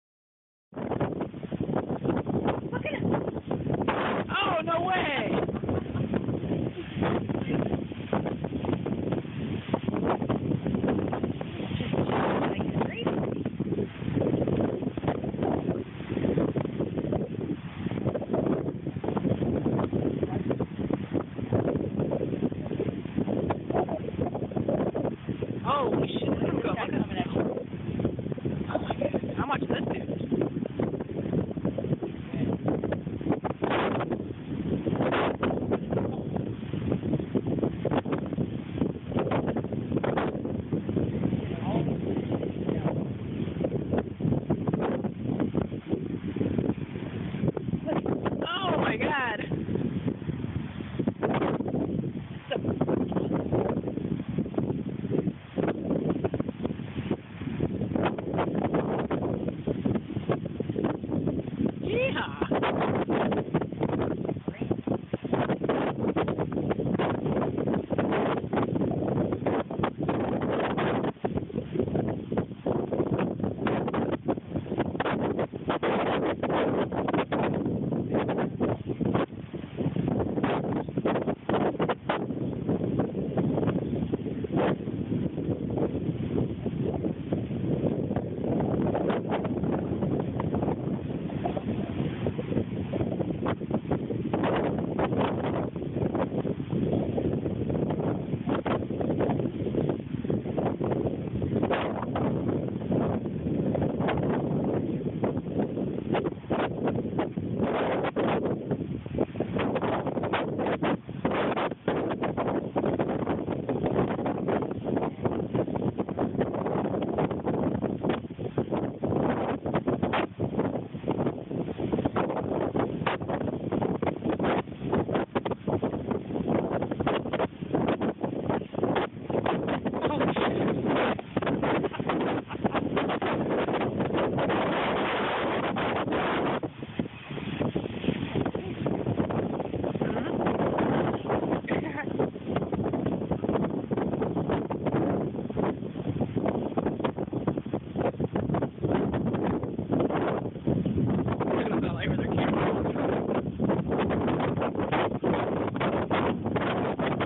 Labor Day Holiday Fun on the Chesapeake Bay, 2010
Fun on Labor Day right outside of our house on the Chesapeake Bay